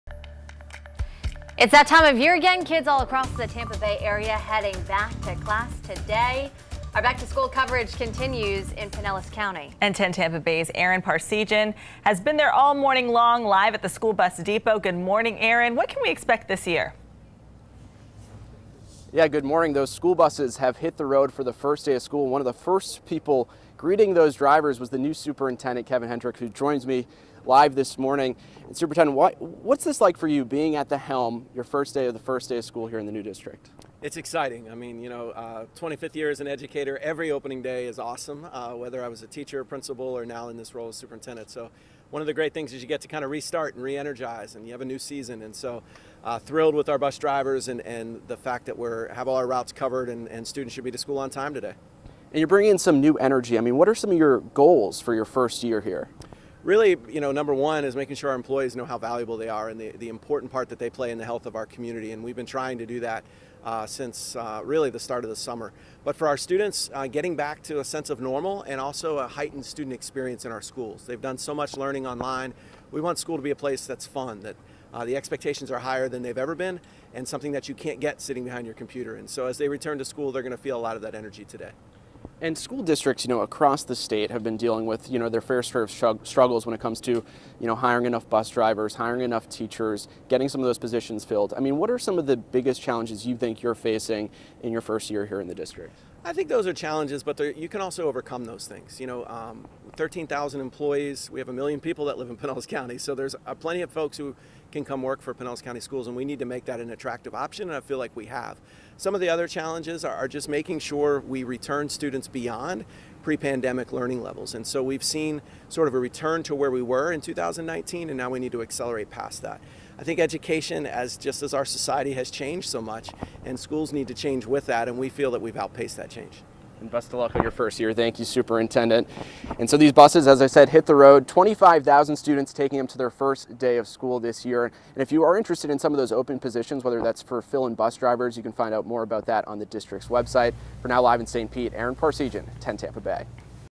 10 Tampa Bay talks with Pinellas County schools' new superintendent
While Kevin Hendricks has been an educator for the past 25 years, it's his first day as the new Pinellas County Schools' superintendent.